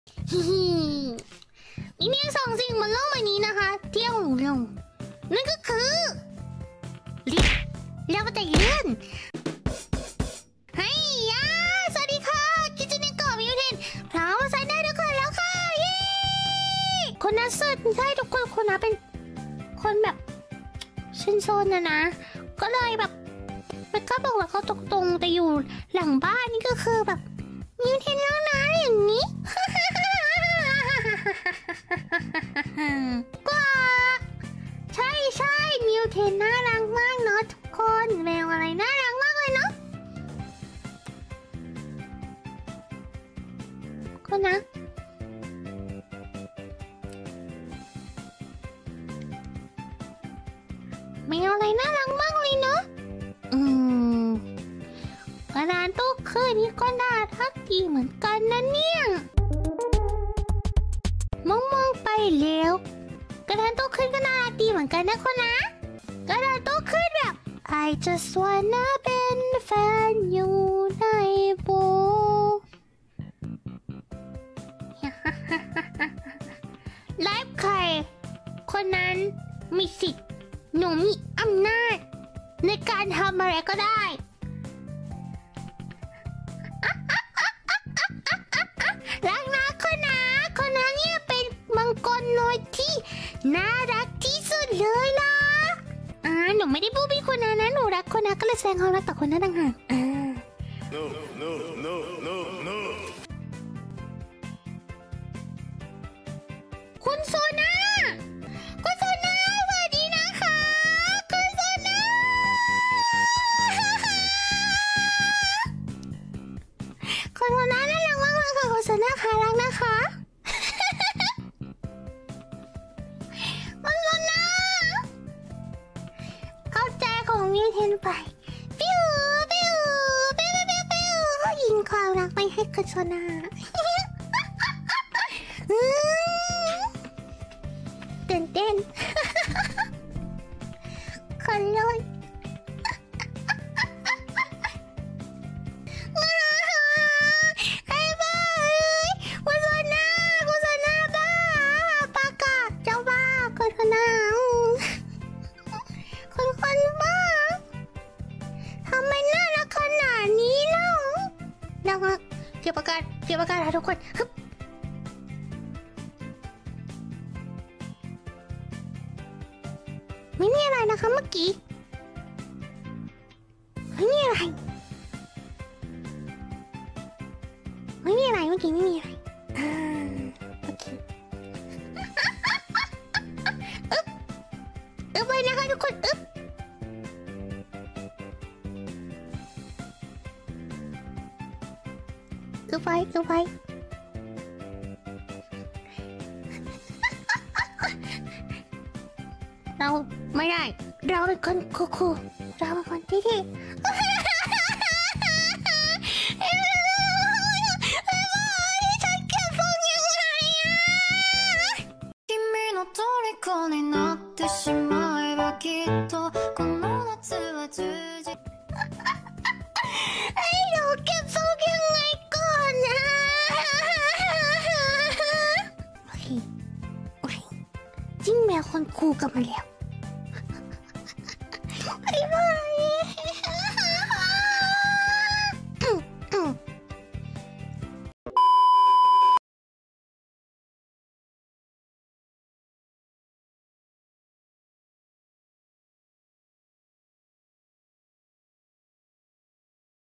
[Vtuber]Hi-light โคนะโดนบู้บี้และมิวเทนเสียอาการต่อหน้าคุณโซน่า
[Vtuber]Hi-light โคนะโดนบู้บี้ และ มิวเทนเสียอาการต่อหน้าคุณโซน่า!!
น้องน่าจะ simp คุณ โซน่า มากอ่ะ ถึงเสียอาการได้เบอร์นี้

ตามไปตำและดูไลฟ์ย้อนหลังได้ที่นี่